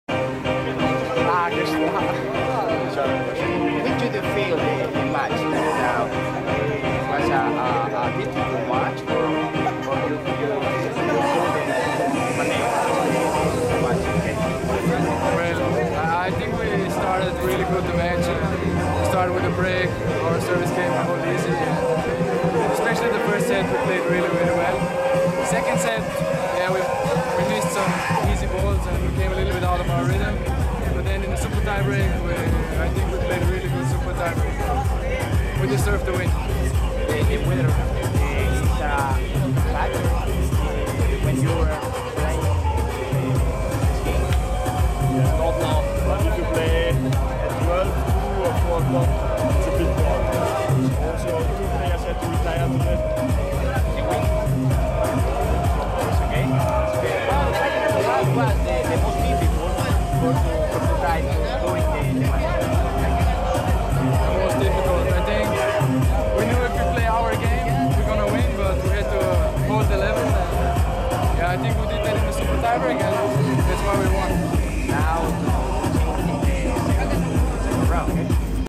Pareja de austriacos
Pareja conformada por Michael Linzer y Gerald Melzer, ganadora del partido de dobles conformada por Diego Hidalgo y Michael Quintero, en la jornada vespertina del lunes 27 de febrero en el Challenger de Salinas 2012